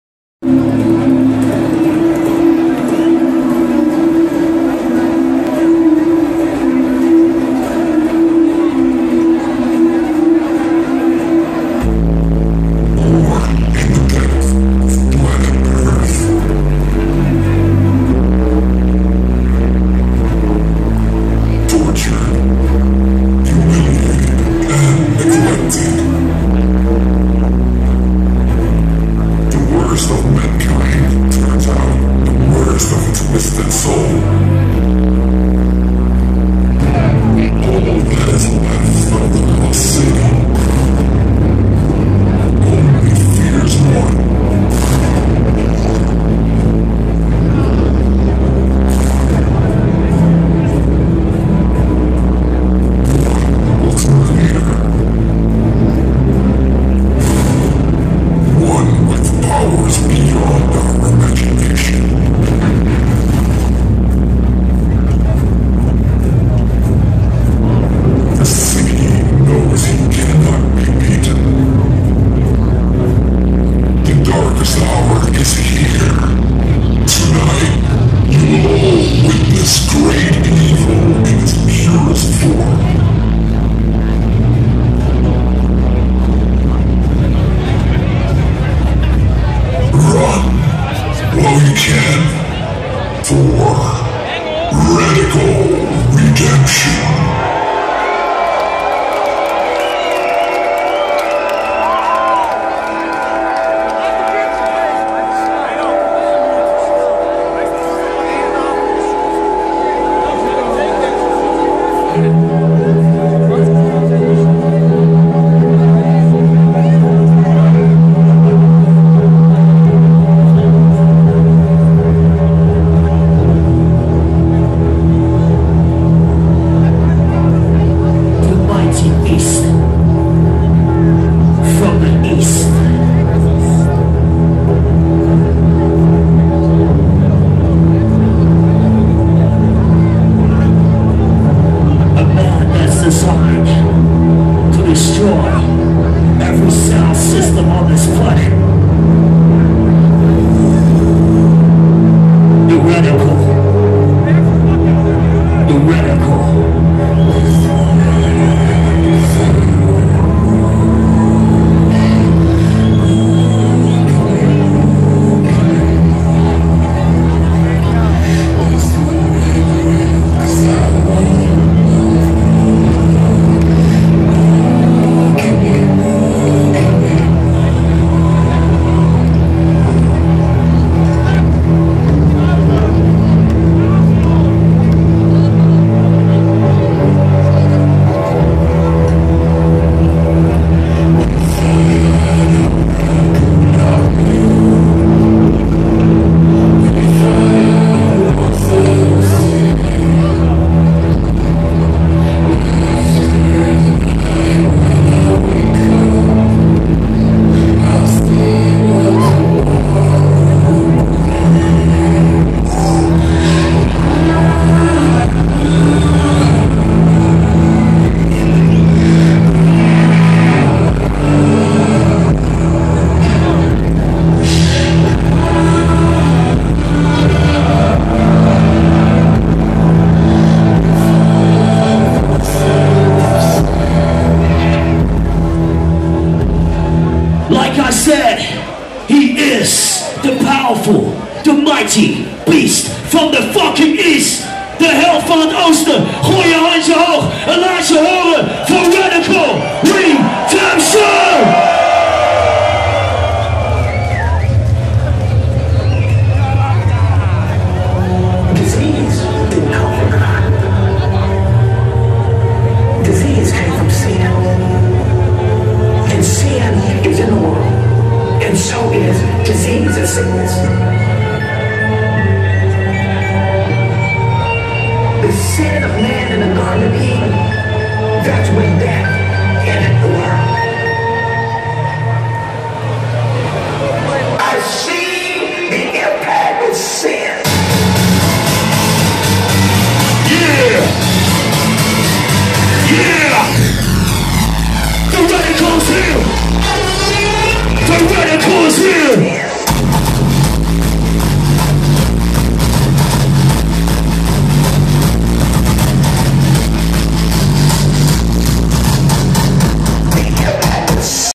Introduction Radical Redemption at Focus
Intro en upcoming of Radical Redemption at Focus in Lucky Rijssen.
6 December